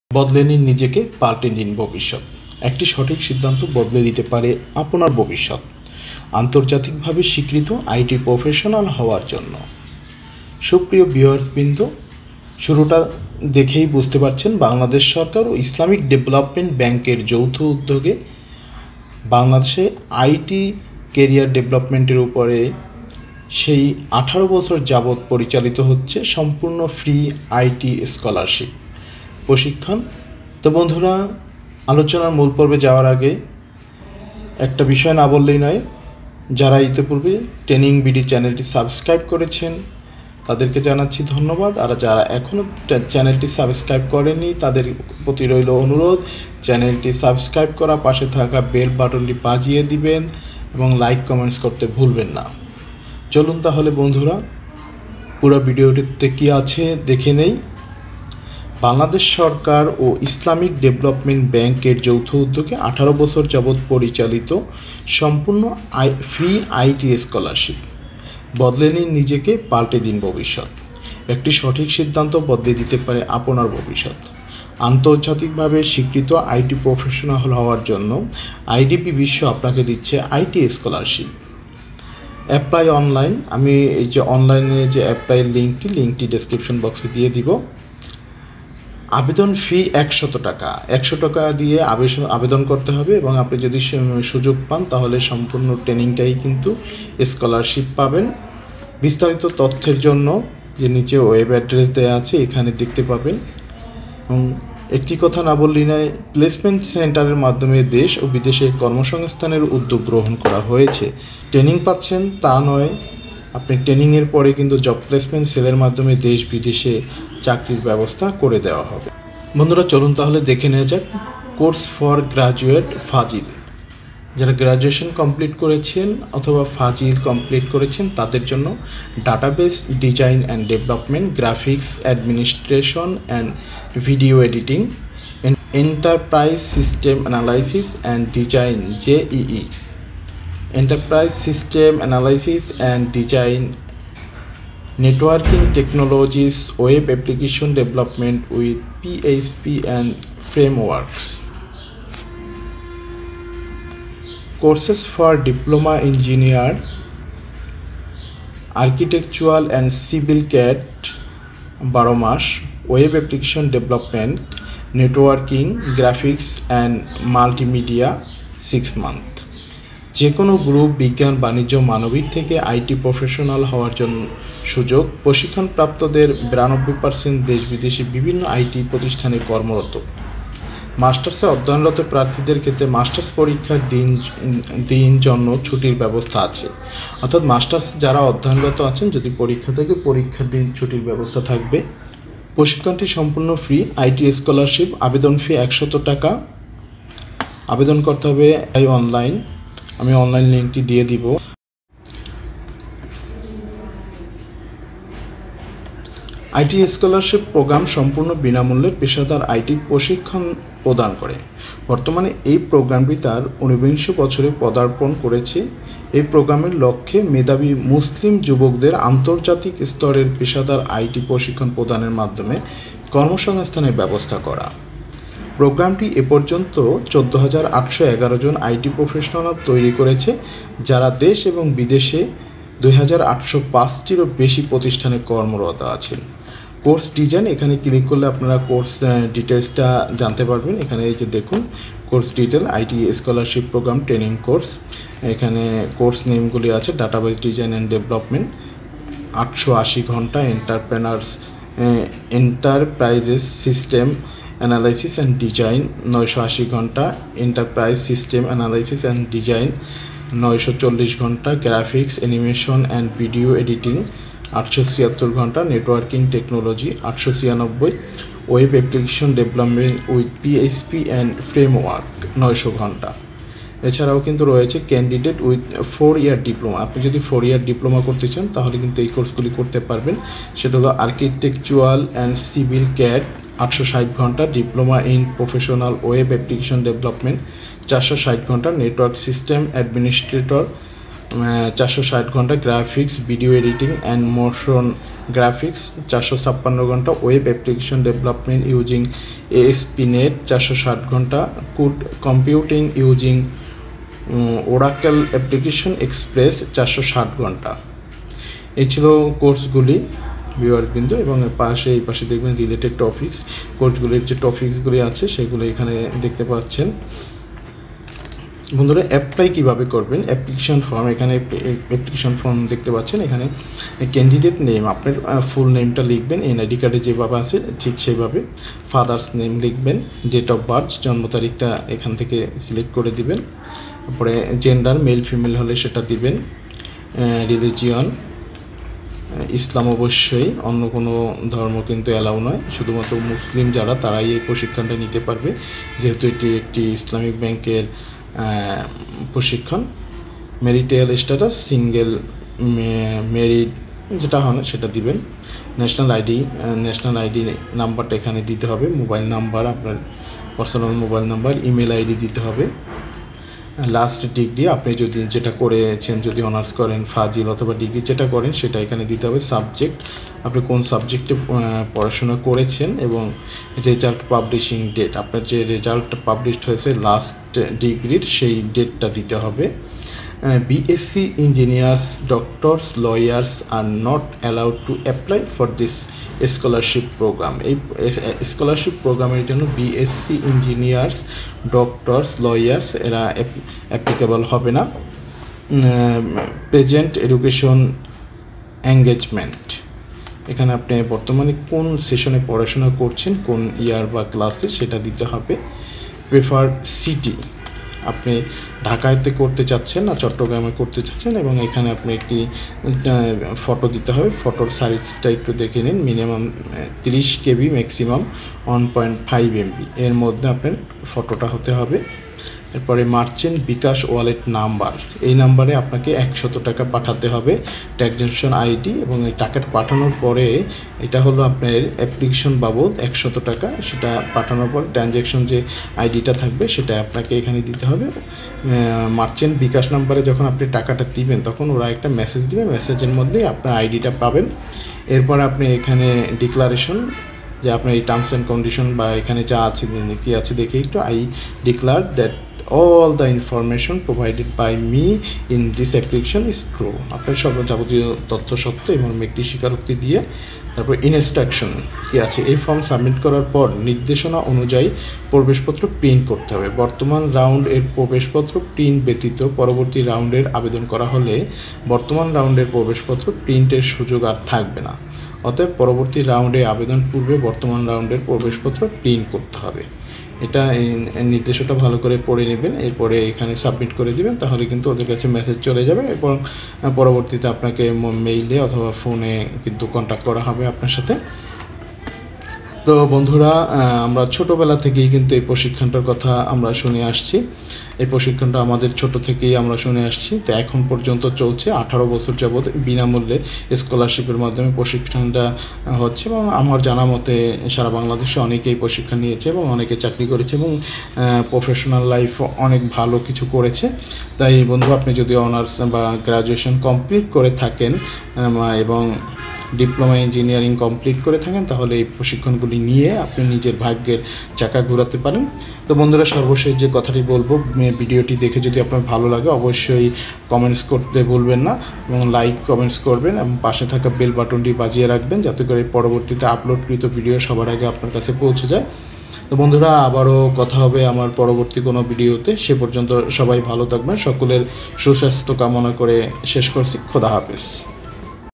IsDB-BISEW IT Scholarship Programme , ফ্রি আইটি স্কলারশীপ
Candidates with Bachelor/Fazil/Master/Kamil
Course name
1. Database Design and Development (DDD)
2. Enterprise Systems Analysis & Design - C#.NET (ESAD-C#.NET)
3. Enterprise Systems Analysis & Design - JEE (ESAD-JEE)
4. Graphics, Animation & Video Editing (GAVE)
5. Networking Technologies (NT)
6. Web Application Development with PHP and Frameworks (WDPF)
Candidates with 4-year Diploma
Course name
1. Architectural and Civil CAD
2. Diploma in Professional Web Application Development
3. Network System Administrator
4. Graphics, Video Editing and Motion Graphics
5. Web Application Development Using ASP.NET
6. Cloud Computing Using Oracle Application Express

আবেদন করার শেষ তারিখ:  ৩০ জুন ২০২২